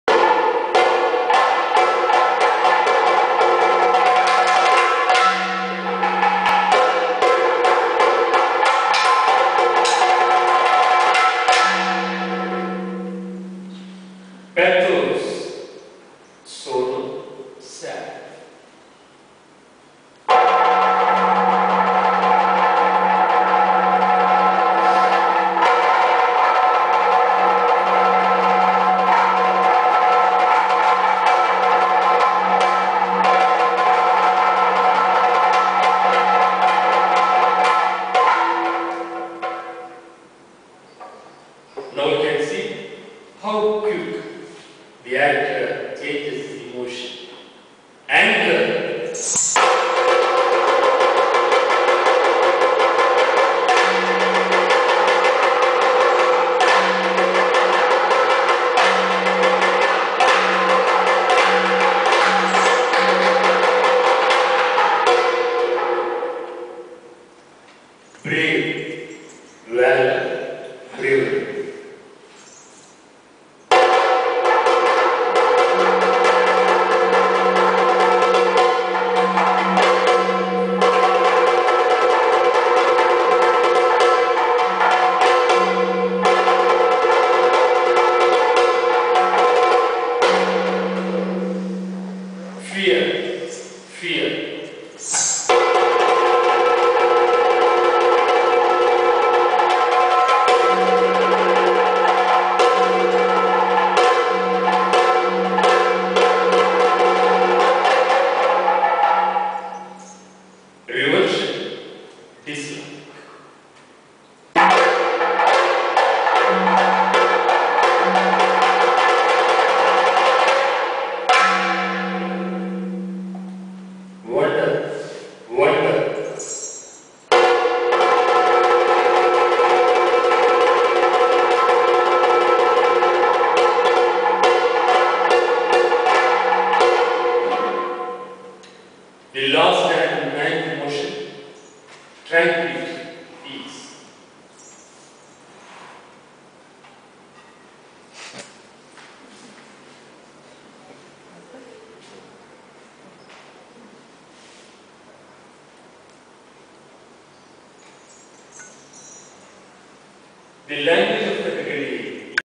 Kathakali - The depiction of the navrasas
A dancer at the Cochin Cultural Centre (Calvetty Road, Fort Cochin) demonstrates the 'navrasas' that form much of the basis of this performing art.